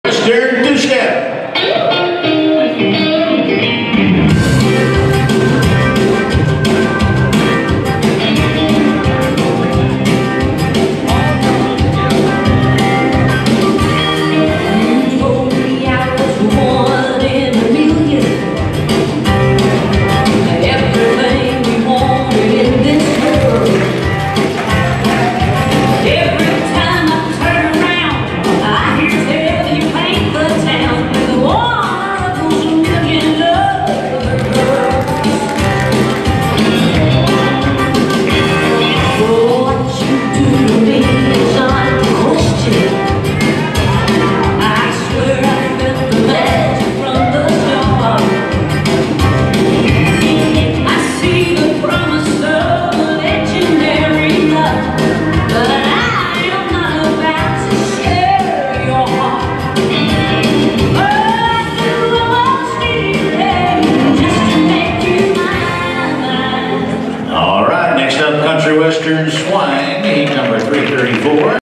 Rabinowitz 333
TwoStep-Sarah and Jarrod-Foxwoods 2019 Arthur Murray Dancearama-Top Score.  6/1/19